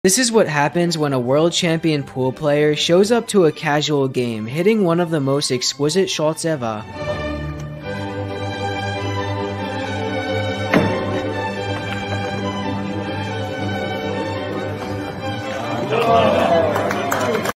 "Masters of the Green: The Perfect Shot"
Dive into the world of precision and finesse as a world-class pool player takes center stage in a mesmerizing display of skill and strategy. In this captivating video, witness the tension mount as the game reaches its climax, with victory hanging on the edge of a single, seemingly impossible shot. Brace yourself for an exhilarating moment as the cue ball glides across the table, executing a flawless stroke that defies the odds and secures the win. It's a masterful display of talent and determination that will leave you on the edge of your seat, marveling at the prowess of a true pool virtuoso. Join us as we celebrate the artistry of the game and the thrill of witnessing perfection in motion.